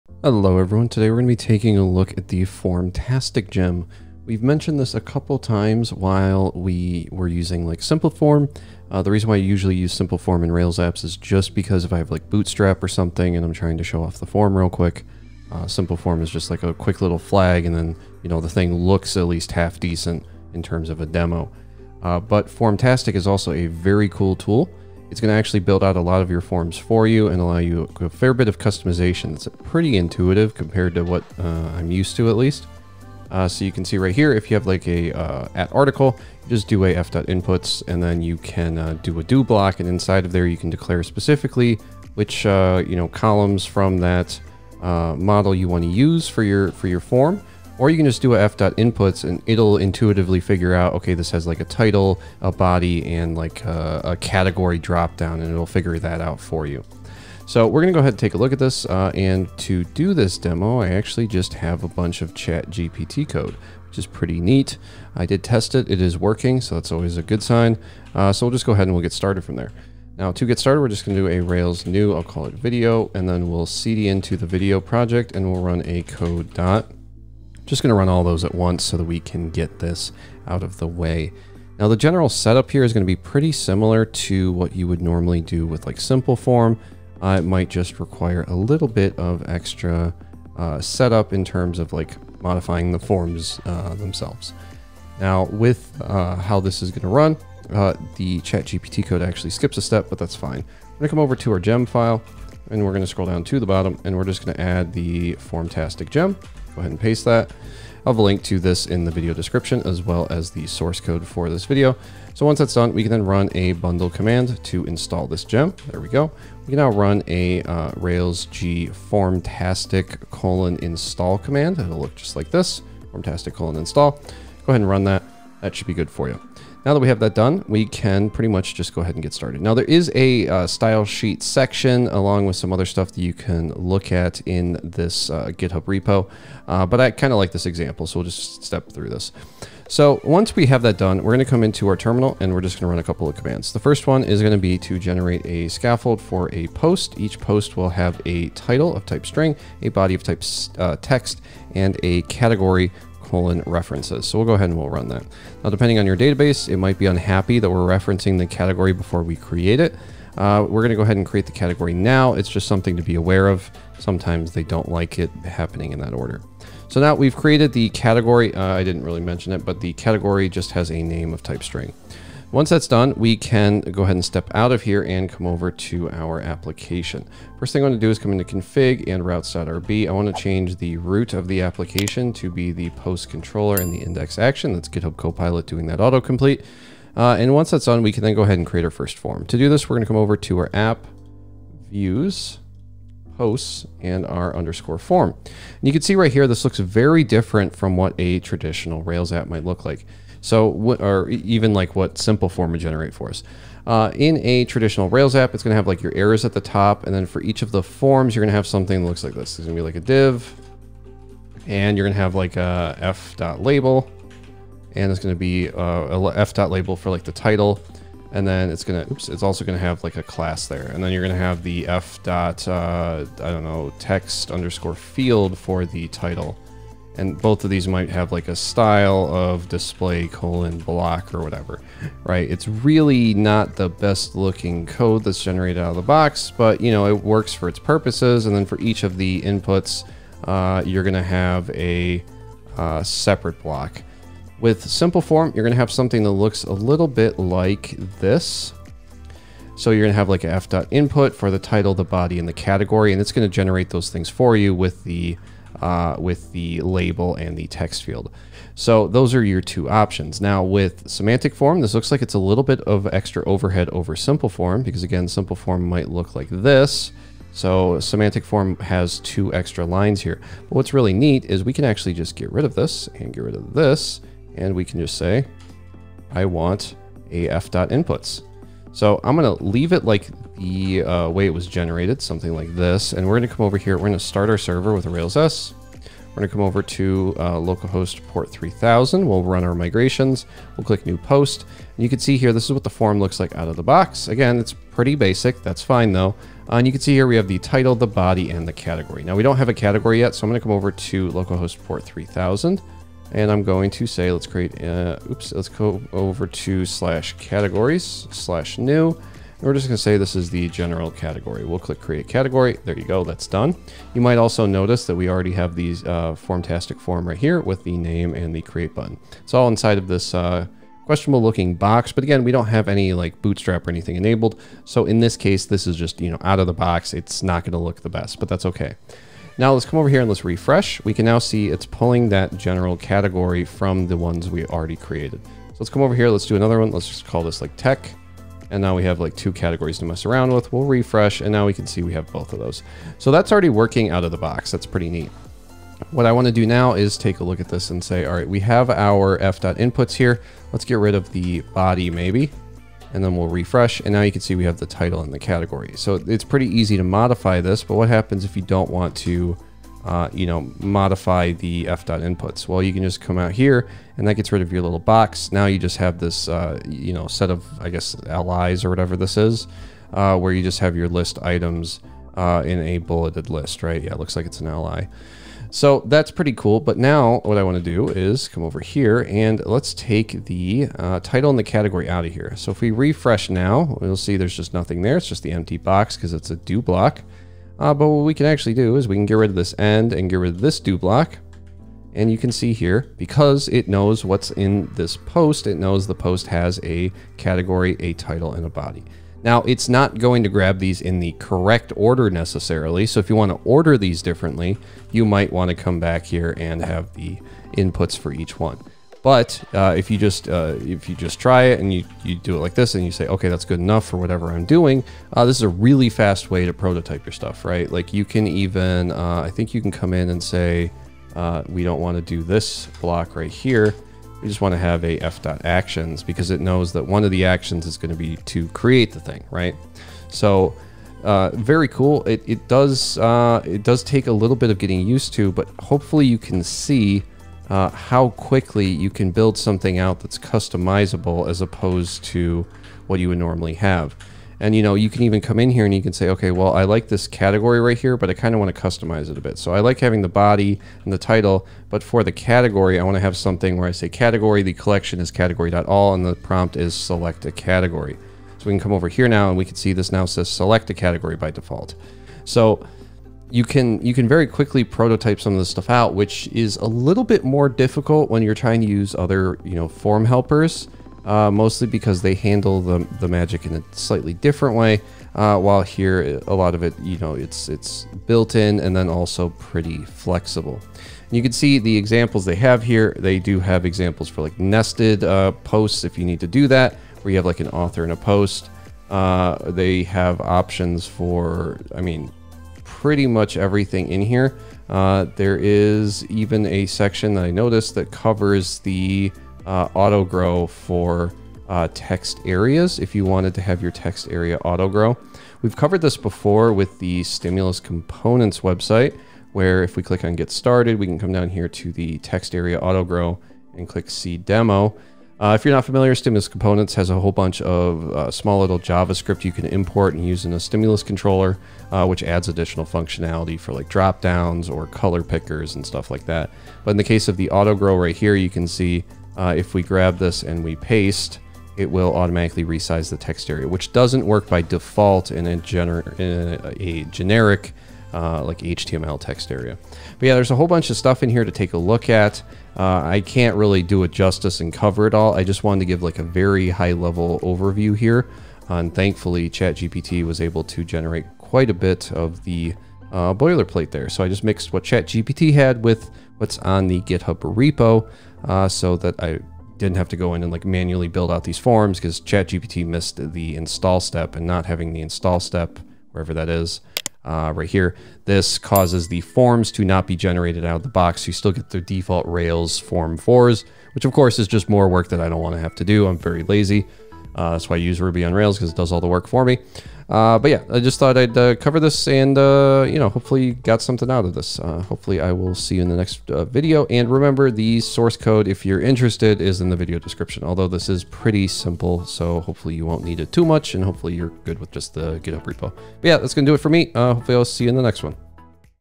Formtastic Makes Forms So Much Easier In Rails 7 | Ruby on Rails 7 Tutorial
Today we take a quick look at formtastic and how it can be used to quickly create some nested forms!